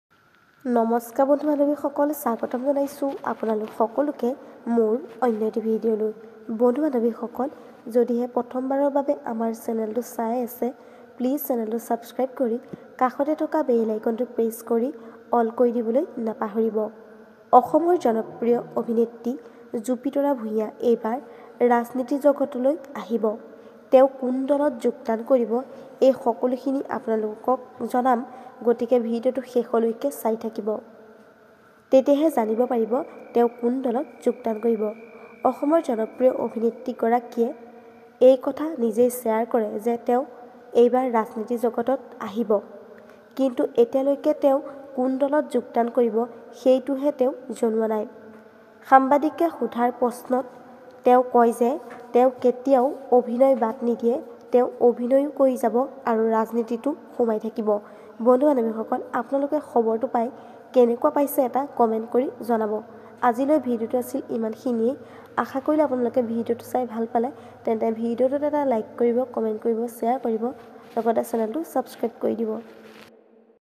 ৰাজনীতি জগতলৈ আহিব জুপিতৰা ভুঞা//নিজেই জনালে সংবাদ মাধ্যমক অভিনেত্রী গৰাকীয়ে//jupitora bhuyan
ৰাজনীতি জগতলৈ আহিব জুপিতৰা ভুঞা//নিজেই জনালে সংবাদ মাধ্যমক অভিনেত্রী গৰাকীয়ে//jupitorabhuyan